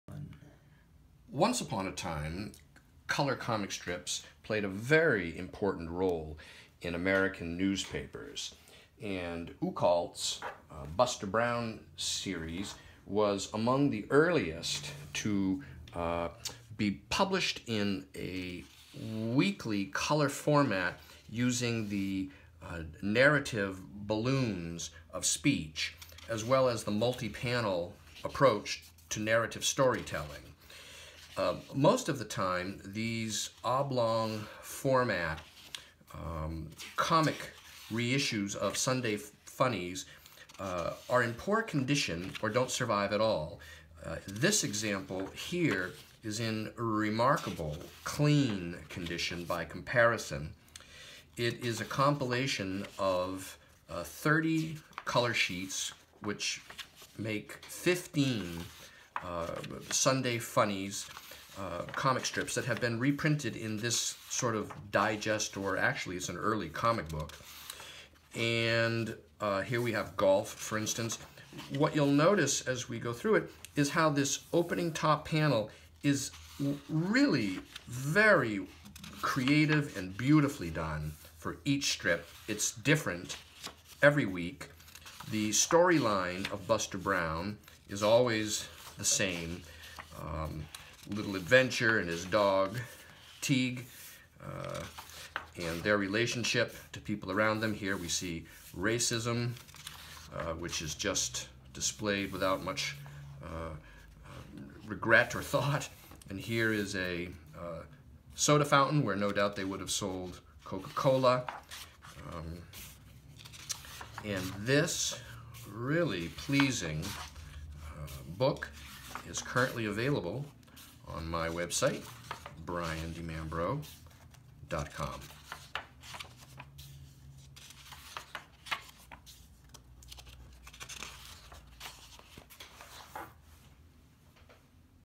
Buster Brown 1905 R.F. Outcault Pranks color comics large old book wonderful
View this wonderful rare oversized collection of early color comic strips by one of the pioneer geniuses of the medium in more detail

Containing thirty (30) full page color lithographed Sunday comic strips, contemporary with their original issuance.

Far nicer than typical condition for this fragile early comic book precursor. Oversized oblong format, thin cardboard covers color lithographed, series of 15 complete Sunday comic strips, each printed across two sheets in splendid oversized format, comprising a total of 30 color sheets plus b&w illustrated title page and covers.

Browse and purchase these and many thousands more antique maps, rare books and prints